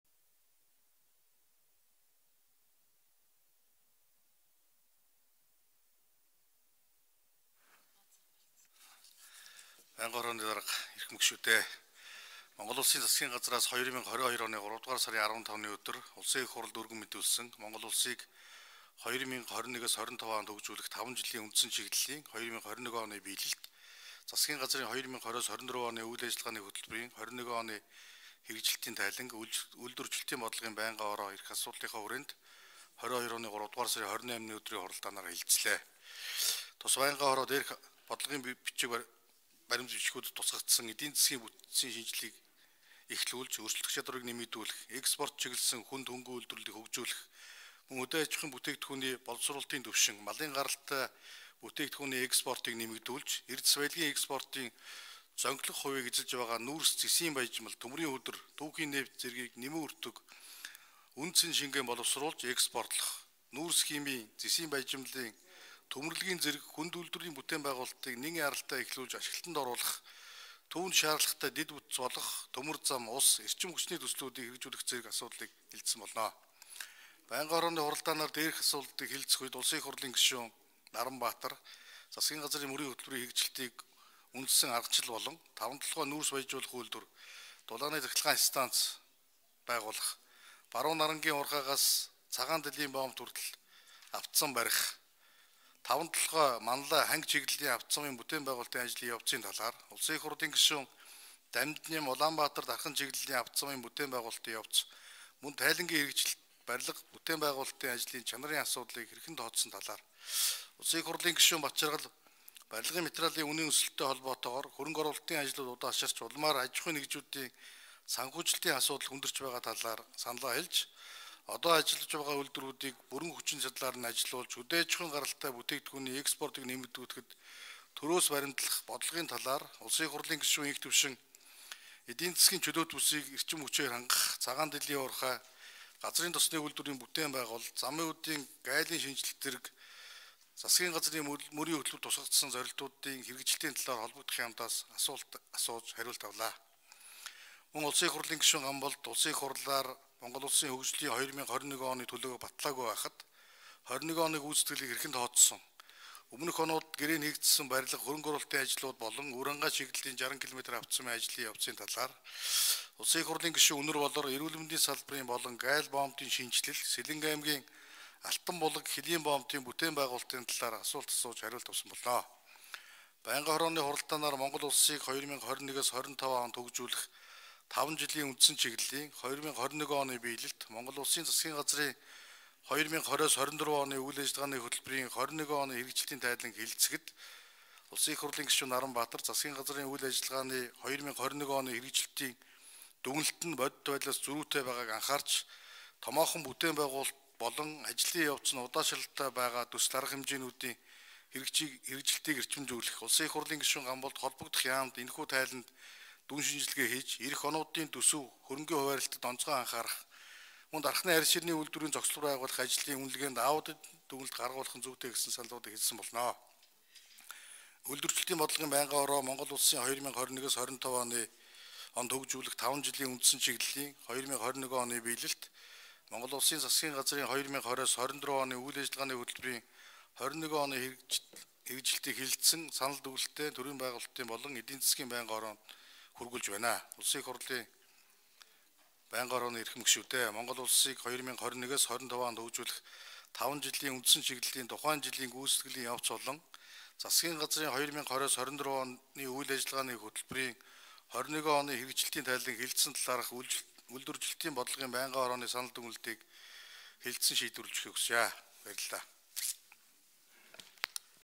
УИХ-ын гишүүн Г.Ганболд Монгол Улсыг 2021-2025 онд хөгжүүлэх таван жилийн үндсэн чиглэлийн ...
УИХ-ын гишүүн Г.Ганболд Монгол Улсыг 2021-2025 онд хөгжүүлэх таван жилийн үндсэн чиглэлийн тухайн жилийн гүйцэтгэлийн явц болон Монгол Улсын Засгийн газрын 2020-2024 оны үйл ажиллагааны хөтөлбөрийн 2021 оны хэрэгжилтийн тайлангийн талаар Үйлдвэржилтийн байнгын хорооны санал, дүгнэлтийг танилцууллаа